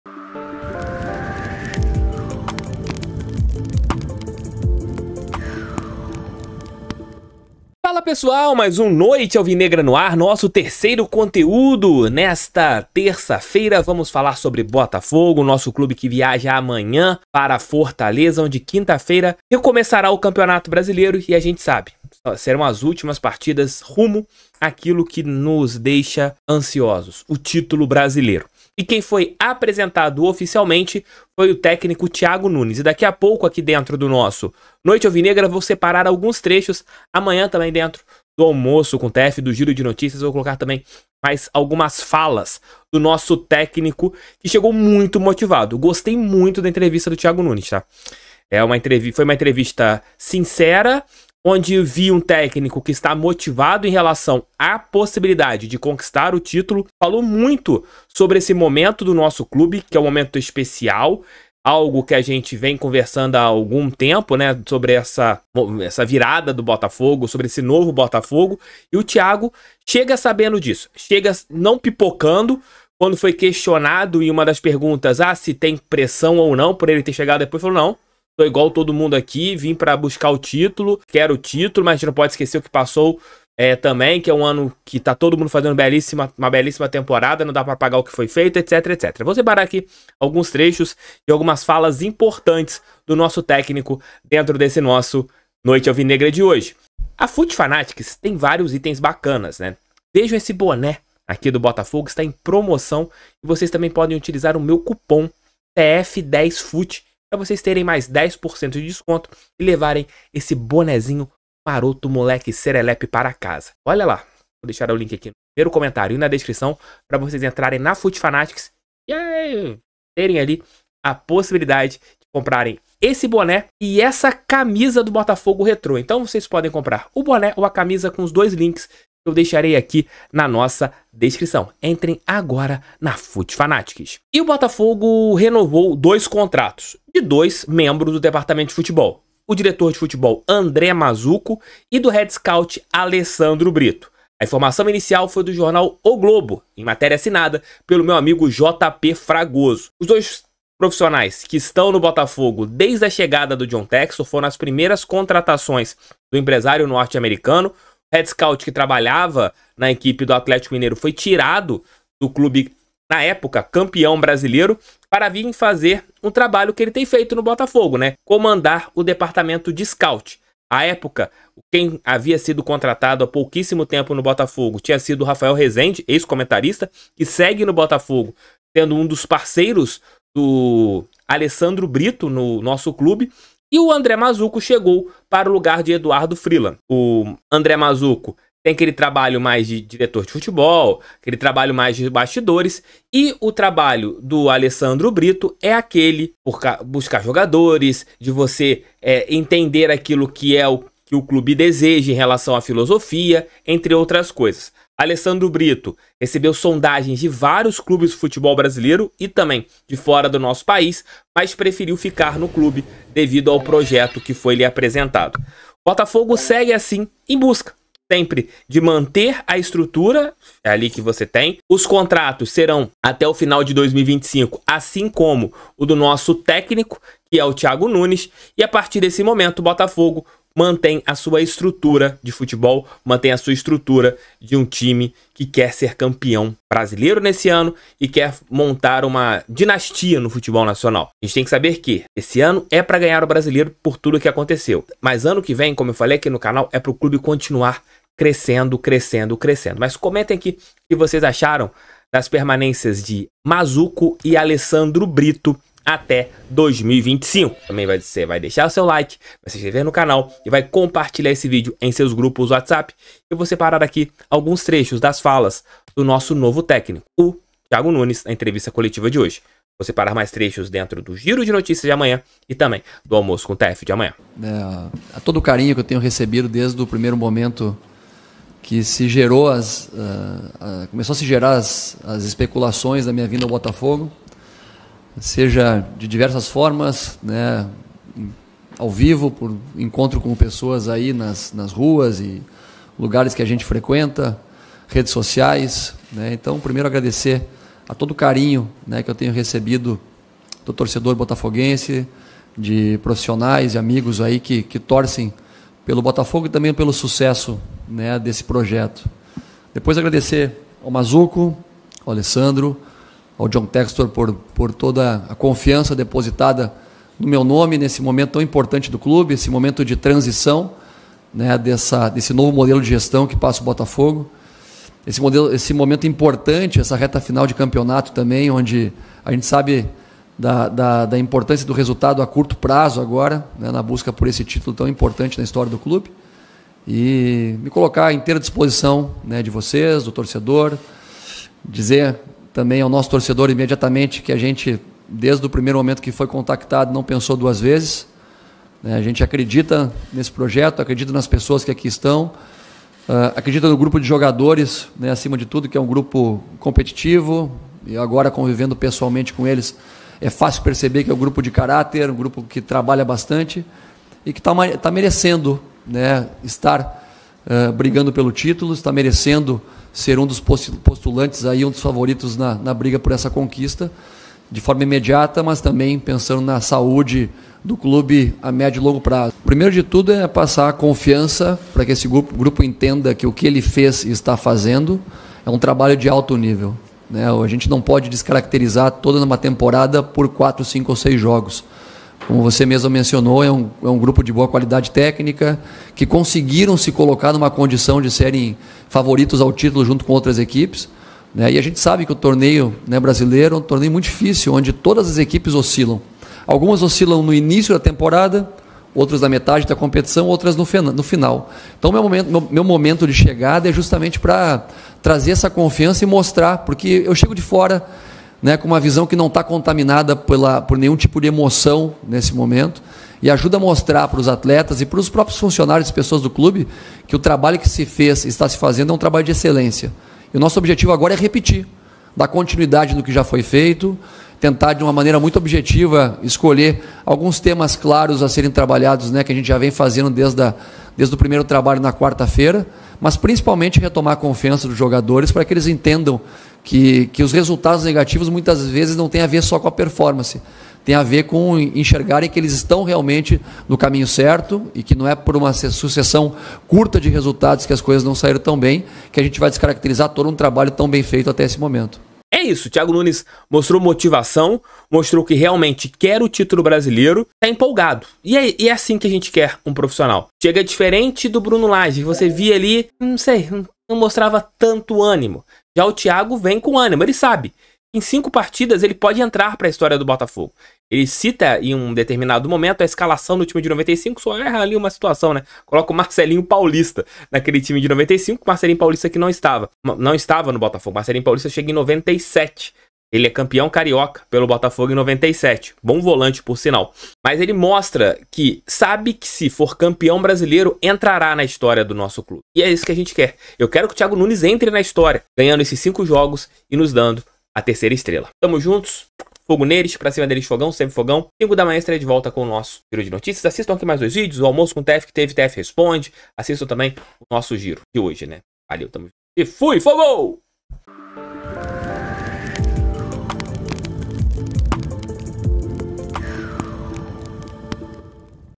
MAZZUCO E ALESSANDRO BRITO RENOVAM CONTRATOS ATÉ 2025 | TIAGO NUNES MOSTRA MOTIVAÇÃO PELO TÍTULO
Leonardo Dantas

Thiago Quintela